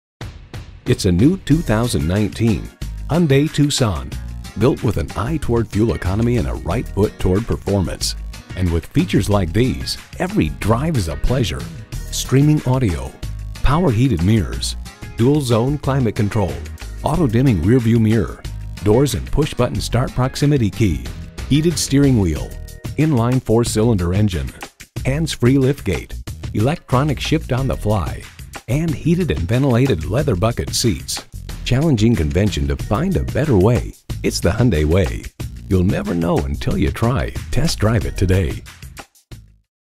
New 2019 Hyundai Tucson Wilkes-Barre PA Scranton, PA K19366 - SOLD
Year: 2019
Make: Hyundai
Model: Tucson
Trim: Ultimate
Engine: 2.4L 4-Cylinder
Transmission: 6-Speed Automatic with Shiftronic
Color: Black
Interior: Beige
Stock #: K19366
VIN: KM8J3CAL0KU056467

Address:
150 Motorworld Dr #3